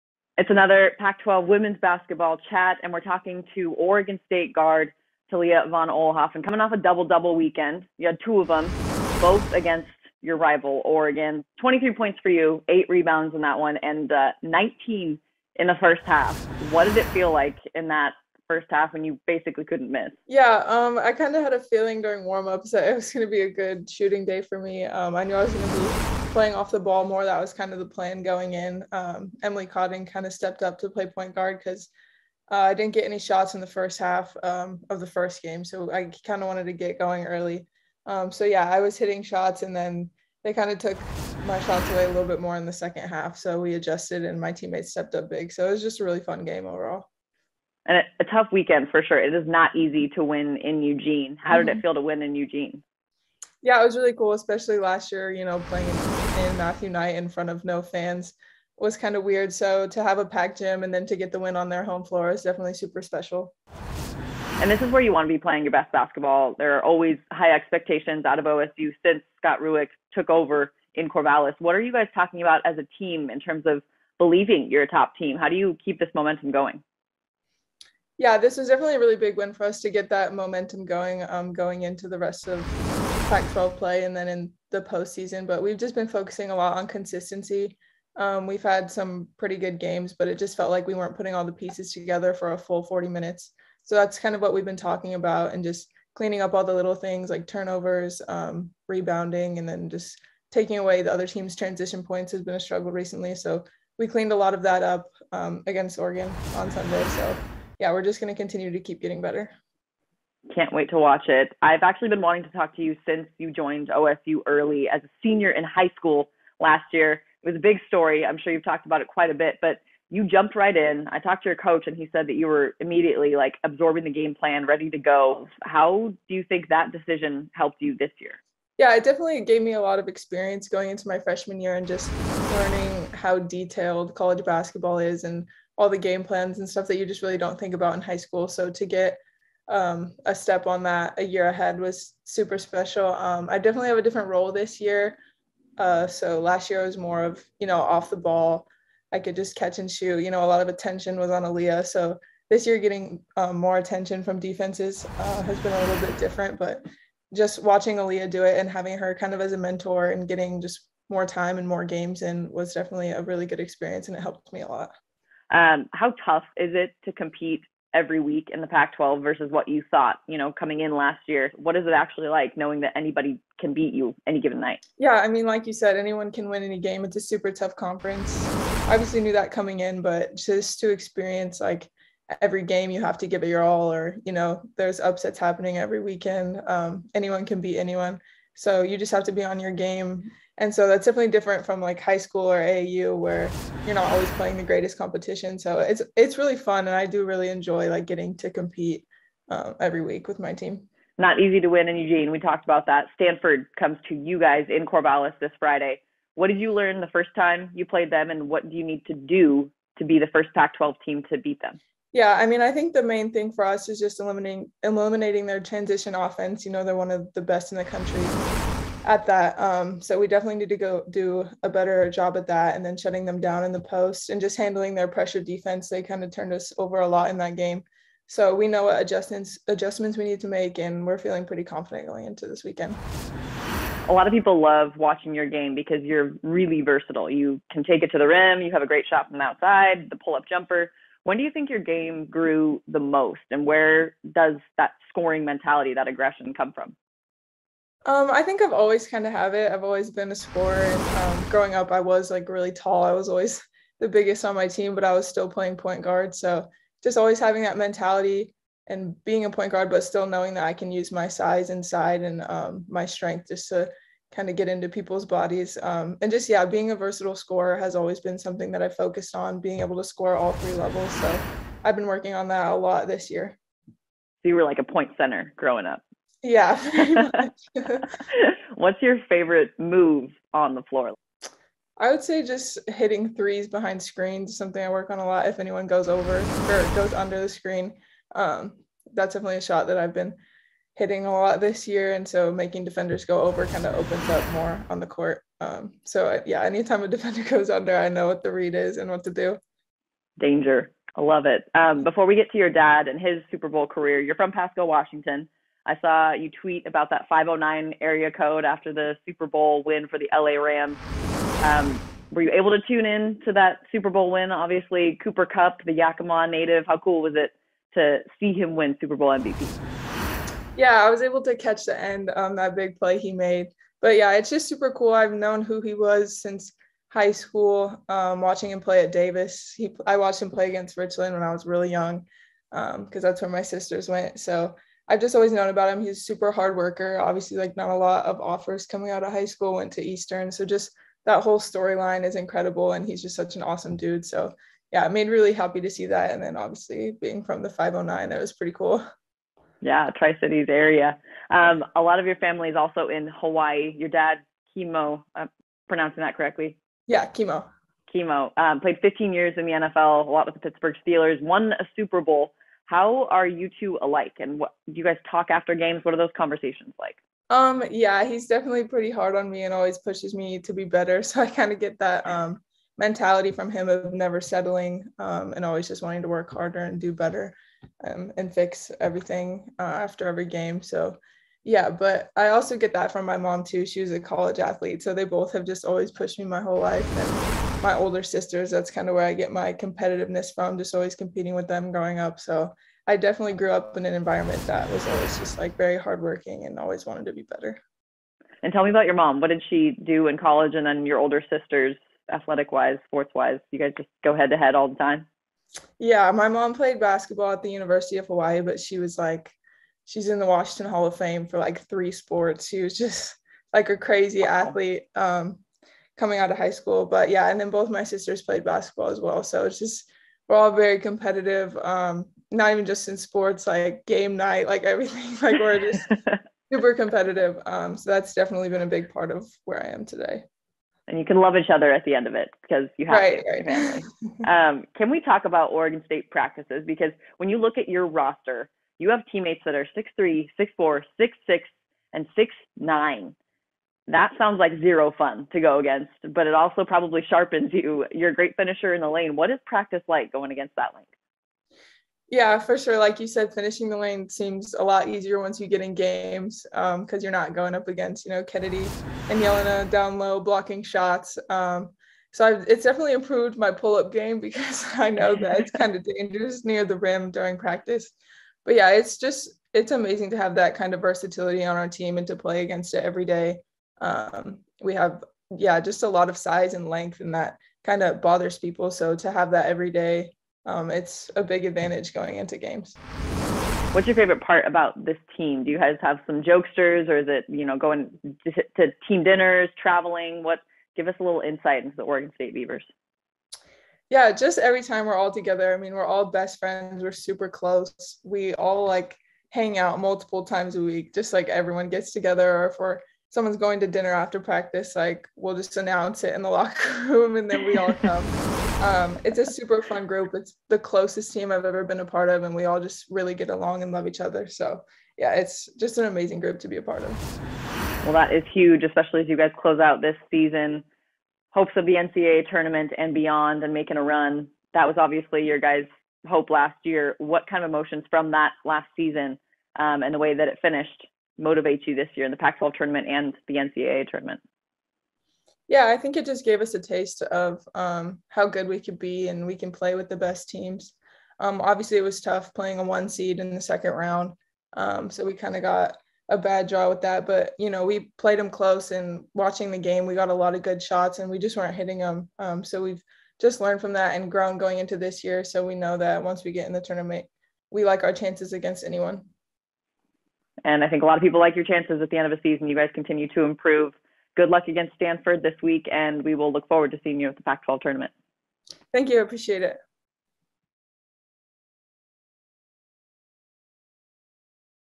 Alyssa Charlston goes in-depth with Oregon State's Talia von Oelhoffen
Alyssa Charlston catches up with Oregon State women's basketball's Talia von Oelhoffen to discuss Talia's big game against Oregon, the Beavers' NCAA Tournament momentum, her decision to enroll early at OSU, and lots more.